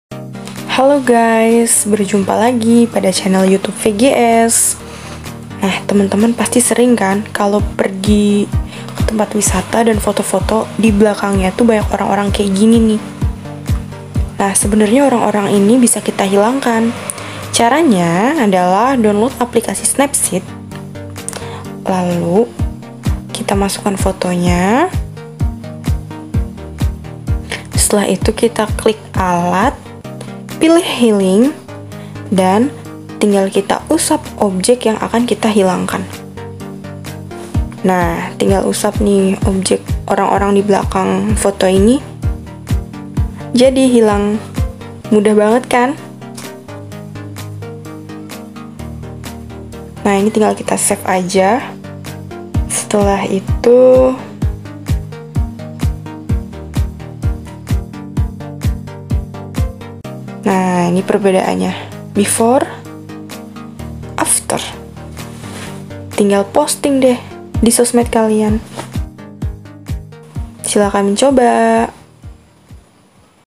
Cara Menghilangkan Objek pada Foto - SIMPEL BANGET
Cara menghilangkan objek pada foto hanya dengan aplikasi Snapseed